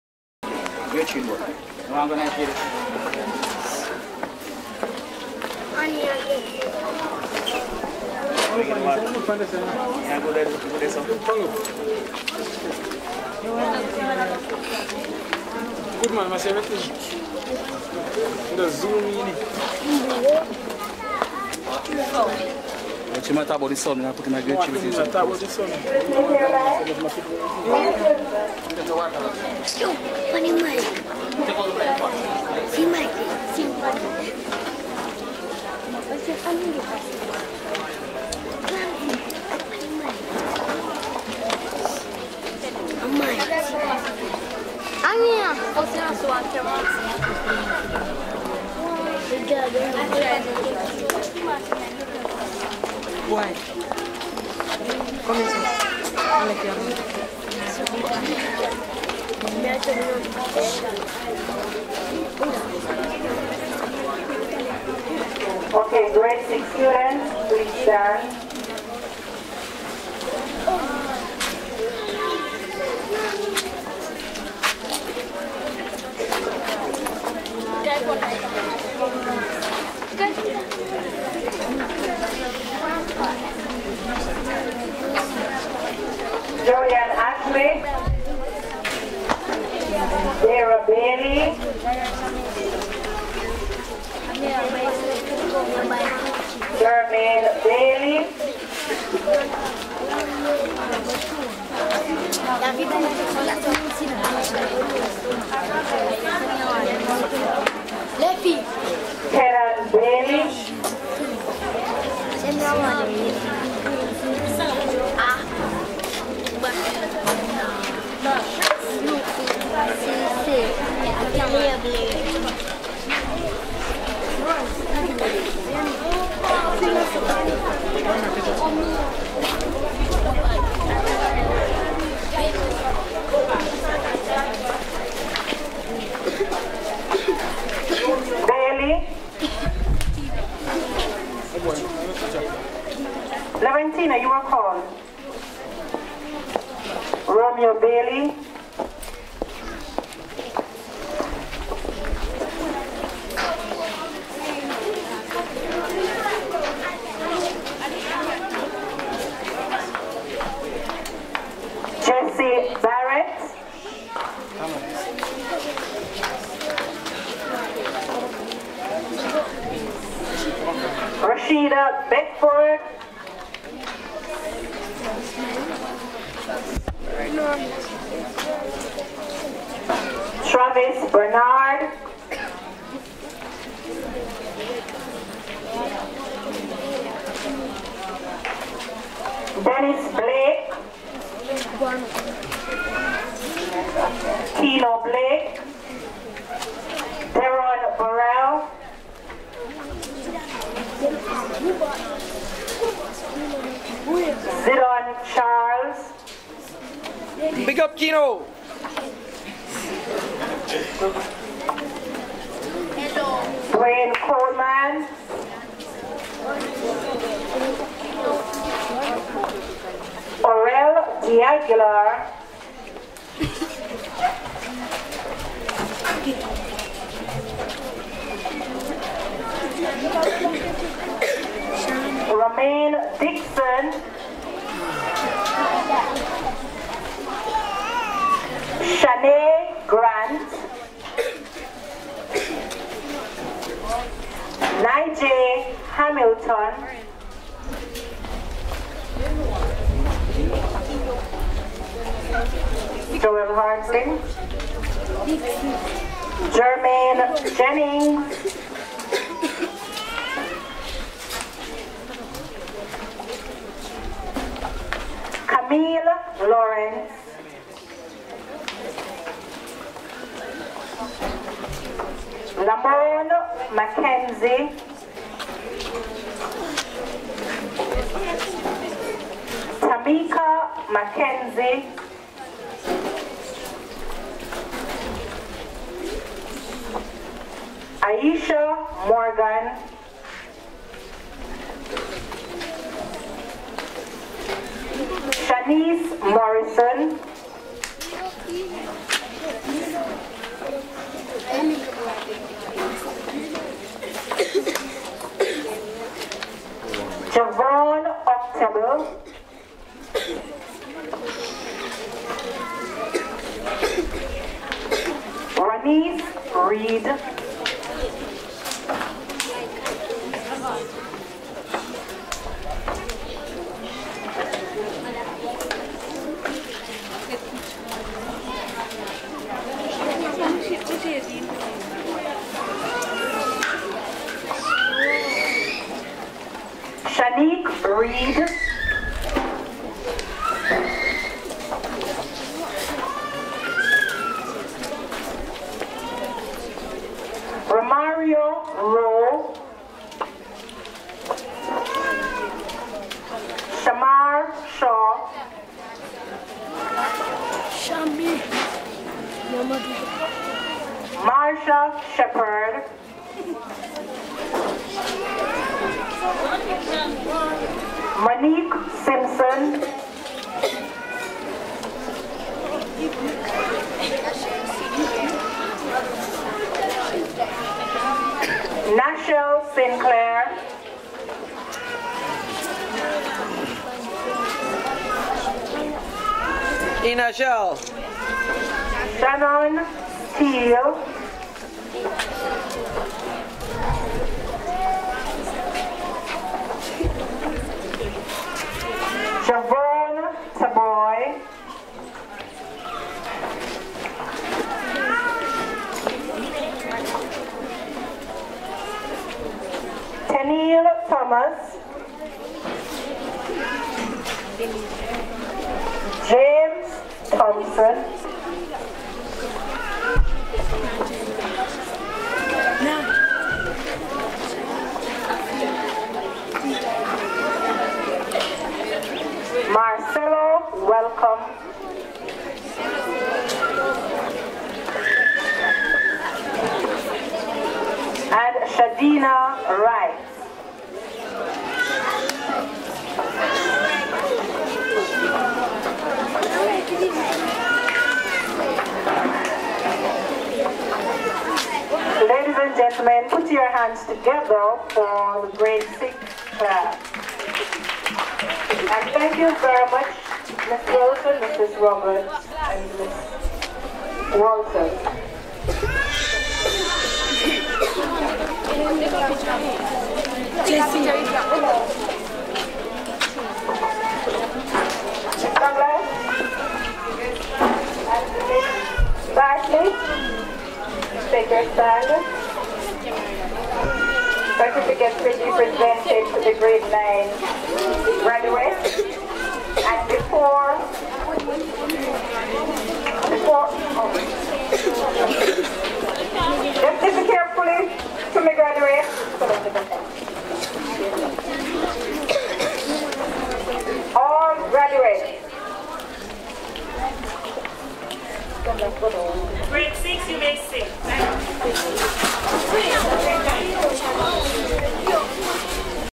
Grad 1.MTS
Elementary School Graduation Ceremony in Rock River, Jamaica